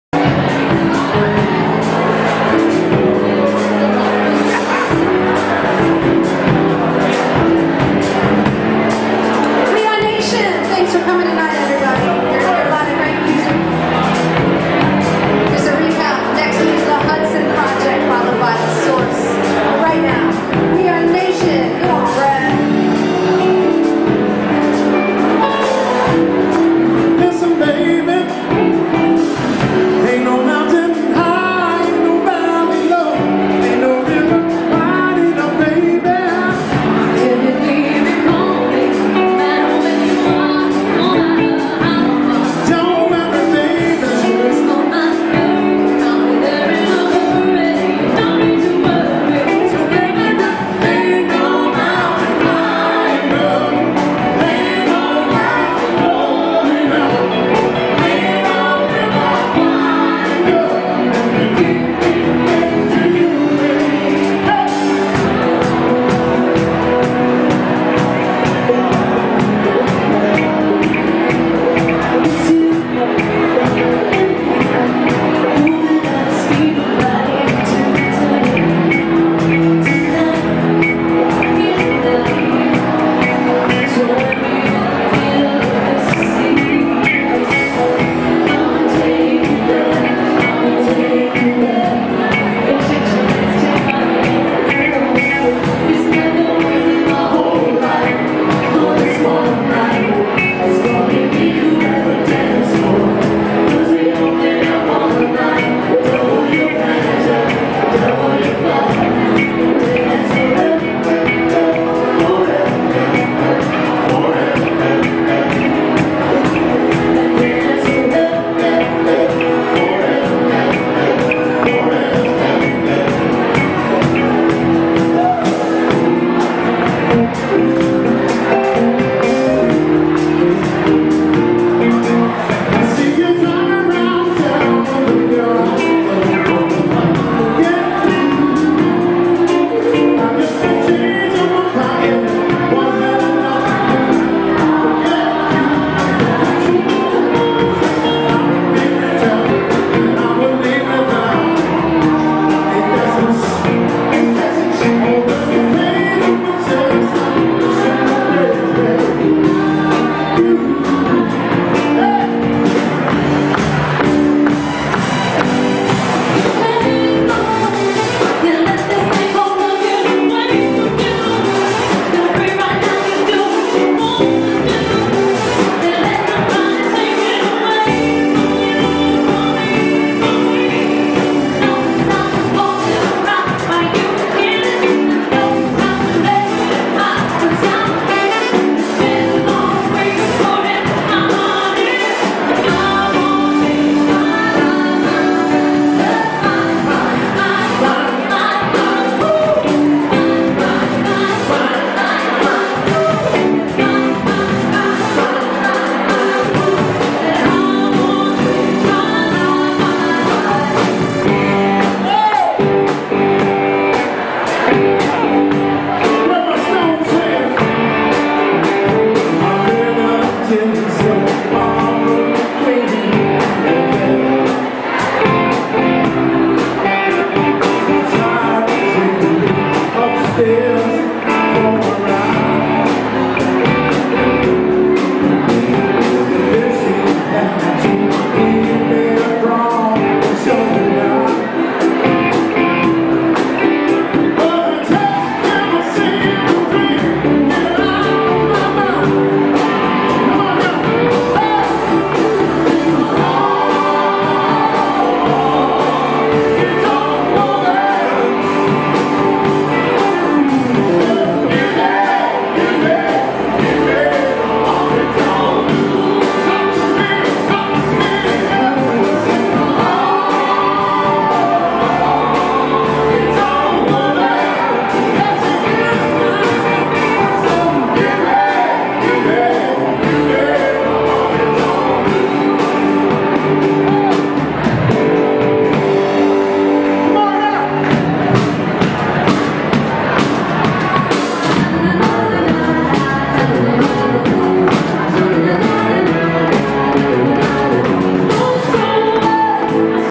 Nation at Halcyon House Georgetown Washington DC
Singing group Nation visits Halcyon House in Georgetown Washington DC for Elan Artists Party